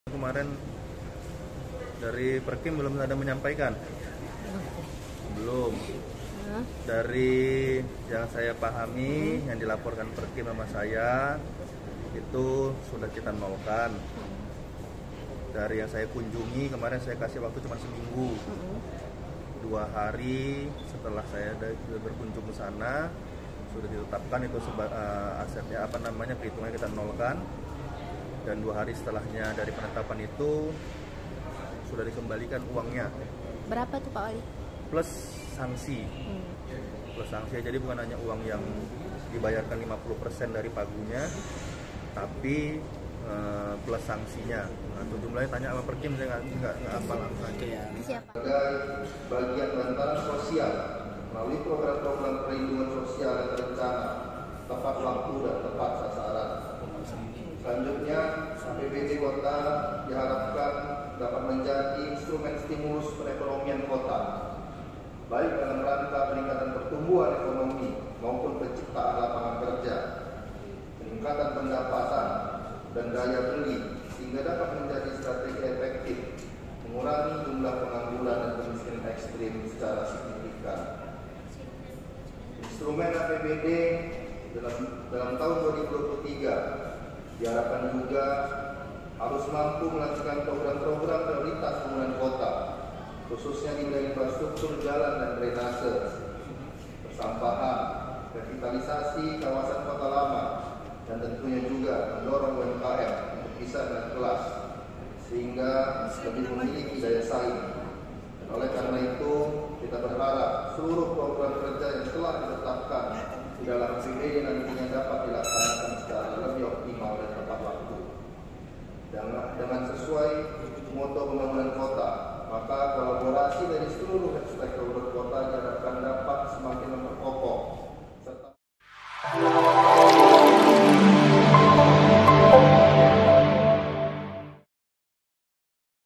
Soal Bangunan Roboh Kejari Medan, Wali Kota Bobby Tegaskan Kontraktor Didenda Pemko Medan
TRIBUN-MEDAN.COM,MEDAN- Wali Kota Medan, Bobby Nasution menegaskan bahwa Pemko Medan sudah mendenda kontraktor yang mengerjakan gedung roboh Kejari Medan.

Namun, Bobby Nasution tidak mengetahui pasti berapa dendanya.

Ia mengatakan, pihak kontraktor juga sudah mengembalikan uang tersebut.